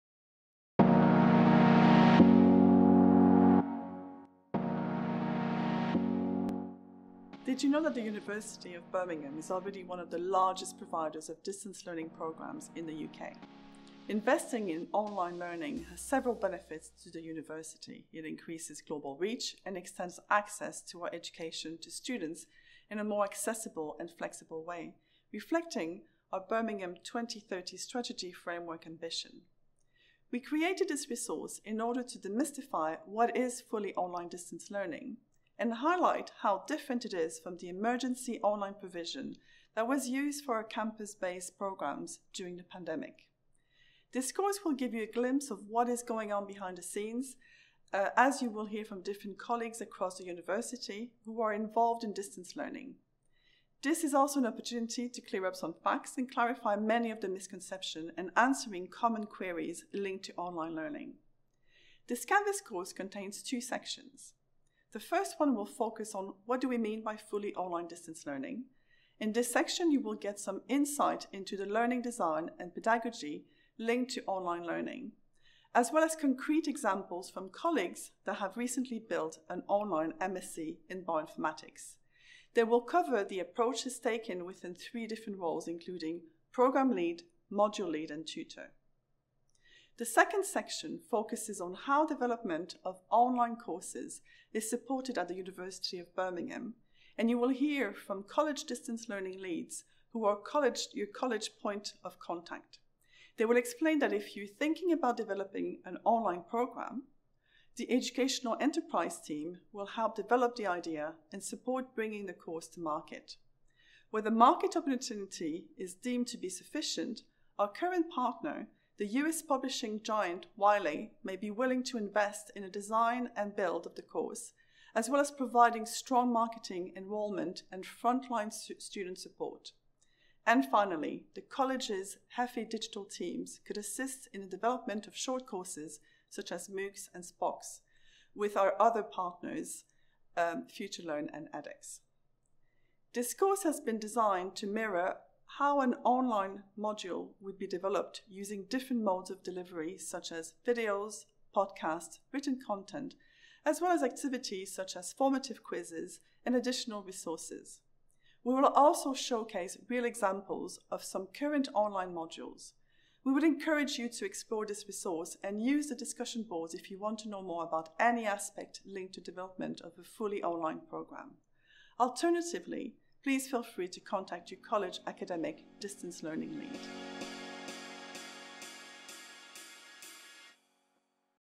MicroCPD: The Reality of Developing Fully Online Distance Learning: Behind the Scenes Facts Checked
Dr Linda Lefièvre (MDS Deputy Director of Digital and Distance Learning) shares a newly launched resource for staff, which dispels some of the myths around developing an online distance learning course; providing a useful overview of the different types of online provision and an insight into the topics covered in the Canvas course.

Explore this online course in order to demystify fully online distance learning. You will hear from colleagues involved in distance learning across the University.